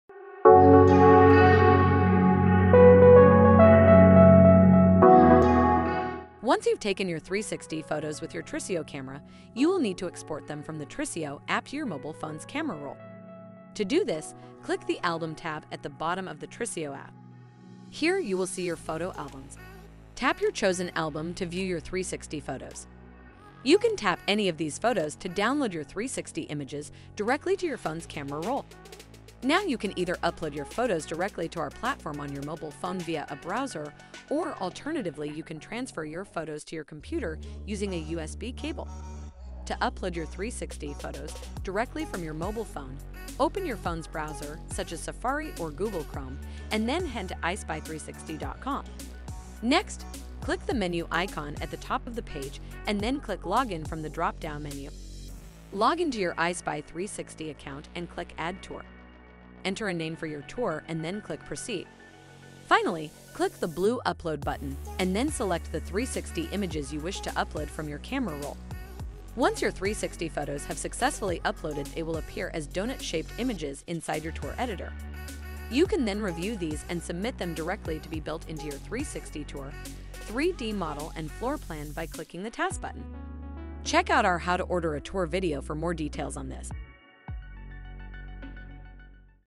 EyeSpy360: How to Export Photos from a Trisio Camera
This video provides a walkthrough guide on how to export your 360 photos from a Trisio camera to your EyeSpy360 account.